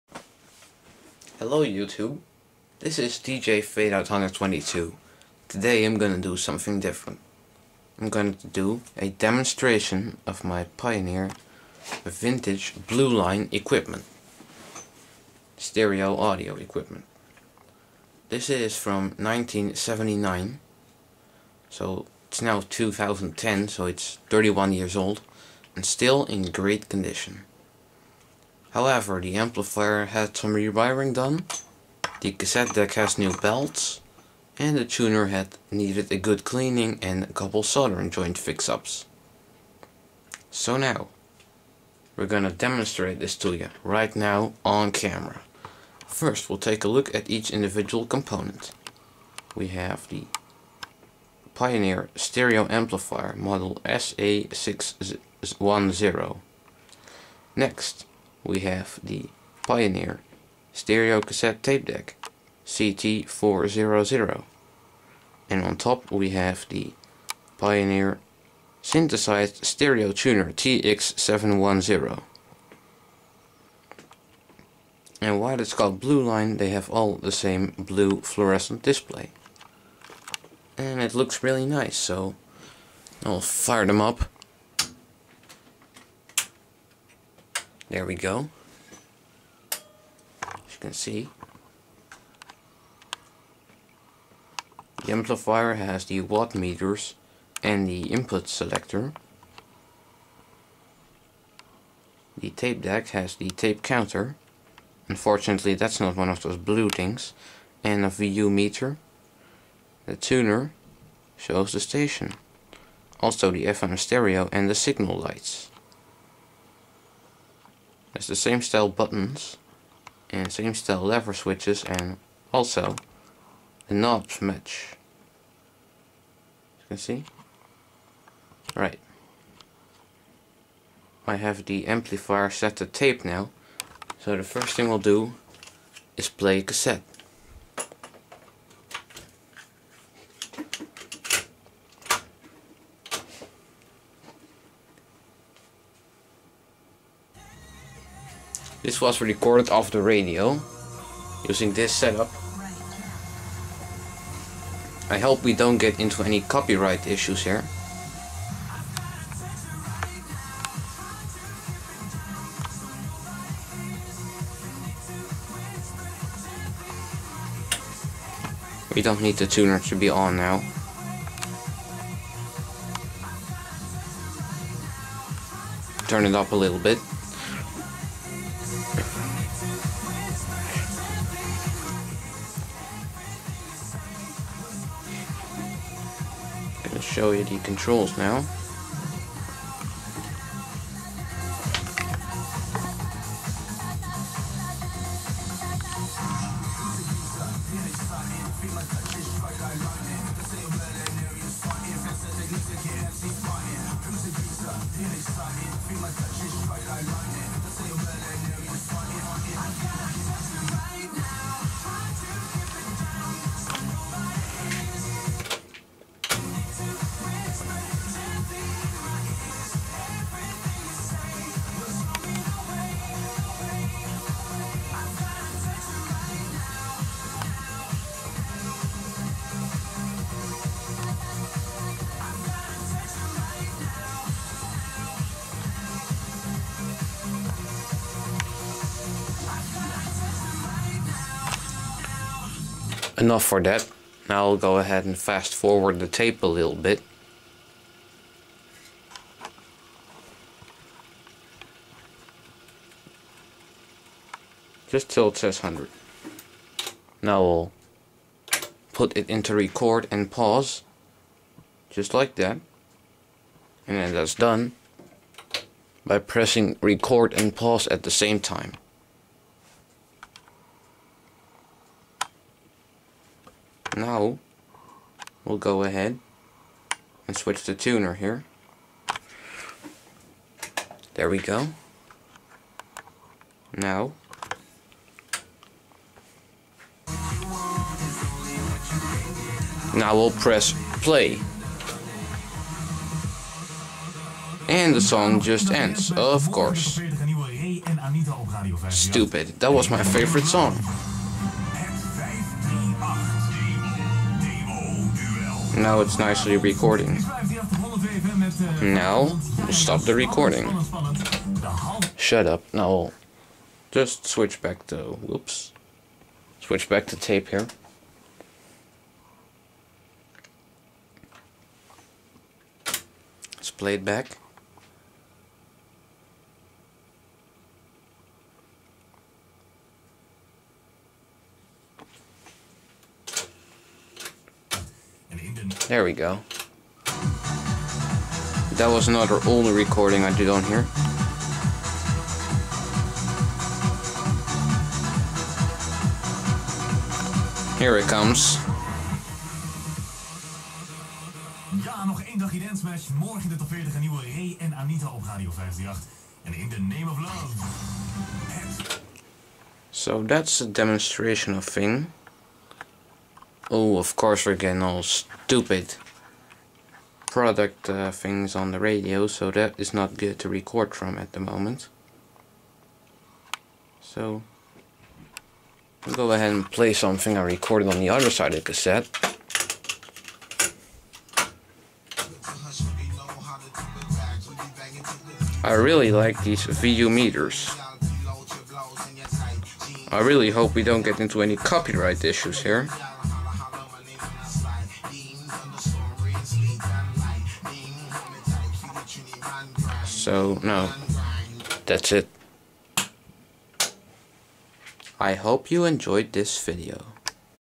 PIONEER Vintage Blue Line Stereo Audio Equipment Demonstation!
This is a demo of my vintage audio setup.
I got the amplifier at a fleamarket for 10 euros.
I got the cassette deck and tuner off ebay for 20 euros each.

I completely rewired the amplifier, and i replaced the belts in the cassette deck.

Parts :

Pioneer Stereo Amplifier SA-610
Pioneer Stereo Cassette Tape Deck CT-400
Pioneer Synthesized Stereo Tuner TX-710

2x JAMO Coronet IV Speakers